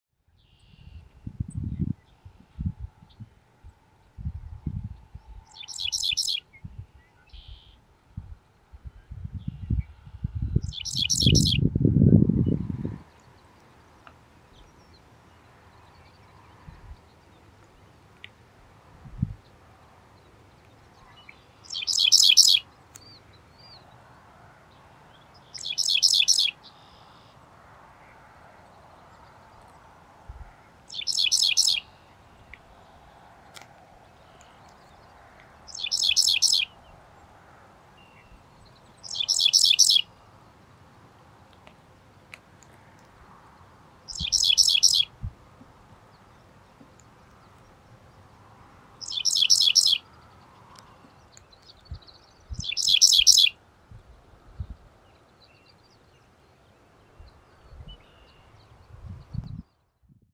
Dickcissel Singing in Geneva, NY
An adult male Dickcissel singing from a farm field along Nielson Rd. in Seneca County, New York on May 25th, 2024. This individual was first located on May 23rd and has been viewed by many birders.